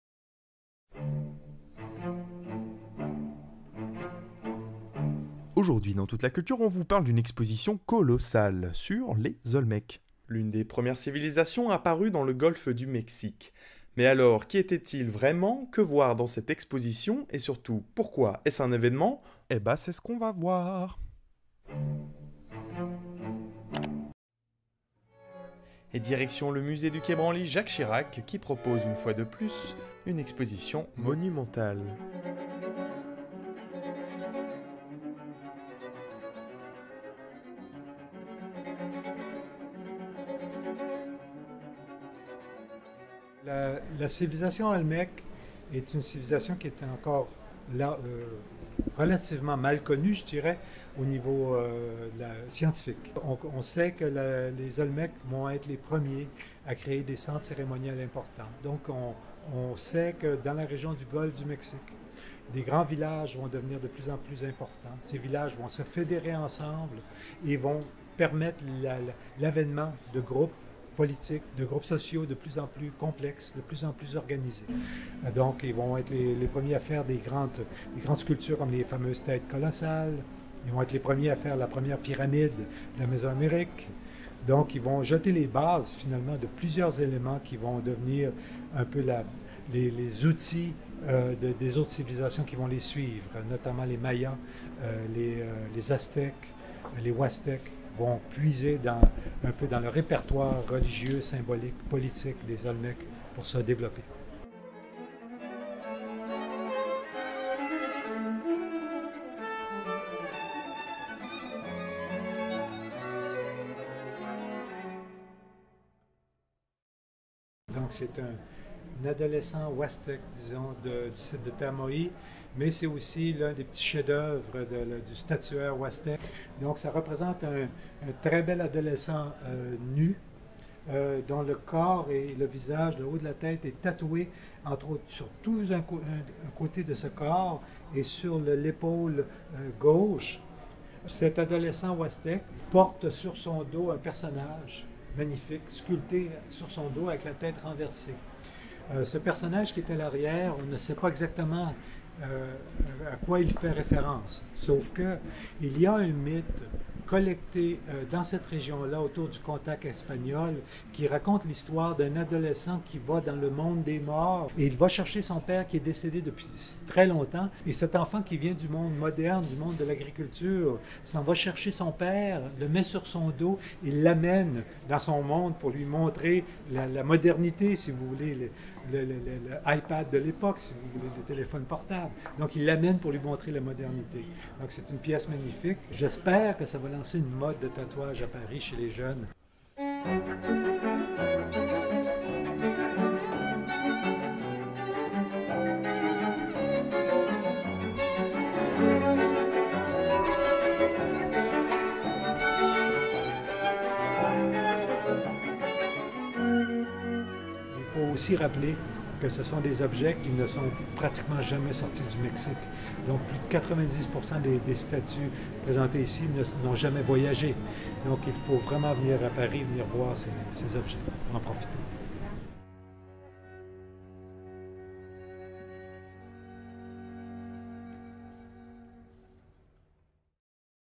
Visite expresse : "LES OLMÈQUES" au musée du quai Branly
Direction le musée du quai Branly – Jacques Chirac pour une visite expresse de l’exposition « Les Olmèques et les cultures du Golfe du Mexique », à découvrir jusqu’au 25 juillet 2021.

Un reportage de Laetitia Larralde et Pierre-Lou Quillard.

🔥  LIENS UTILES
▸ Plus d'informations sur l'exposition et sur la réouverture du musée sur le site du musée :

👍 CETTE VIDEO VOUS A PLU ?